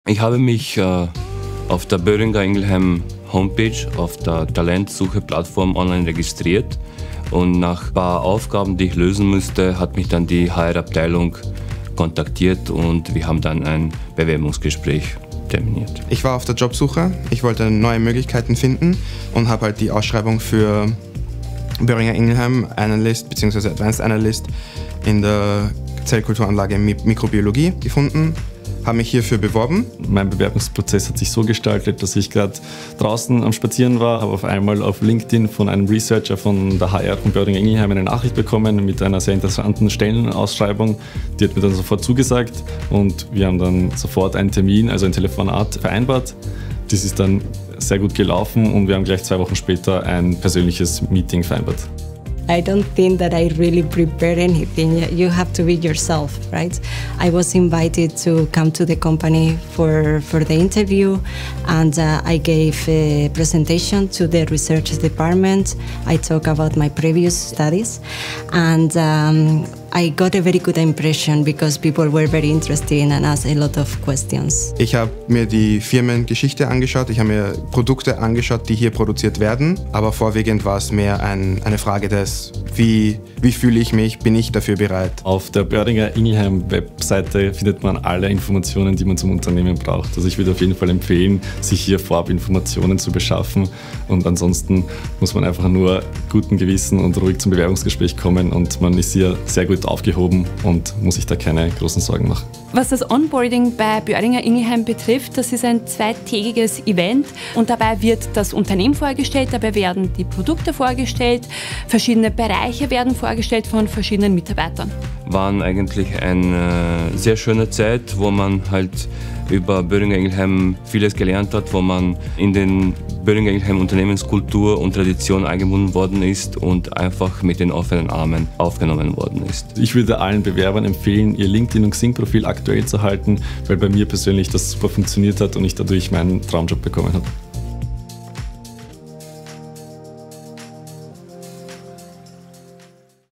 Im Bewerbungsgespräch bei Boehringer Ingelheim RCV GmbH & Co KG überzeugen | karriere.at
Mitarbeiter erzählen, wie sie beim Bewerbungsgespräch bei Boehringer Ingelheim RCV GmbH & Co KG punkten konnten.

Bei Boehringer Ingelheim gibt es eine eigene Online-Talentplattform, bei der sich Arbeitssuchende registrieren können. Passende KandidatInnen werden seitens HR auch direkt über unterschiedliche Plattformen kontaktiert. Zukünftige BewerberInnen sollten sich vorab über die Firma und deren Produkte informieren. Für neue MitarbeiterInnen gibt es ein 2-tägiges Onboarding-Event, wo ihnen die Firma nähergebracht wird.

Der karriere.reporter von karriere.at bei Boehringer Ingelheim RCV GmbH & Co KG:
Interview mit:
Denis - Automation Engineer / Engineering
Leticia - Principal Scientist / Forschung
Michael - Advanced Analyst / Quality
Caroline - Junior Data Manager / Human Pharma
Nedzad -  Manufacturing Operator / Produktion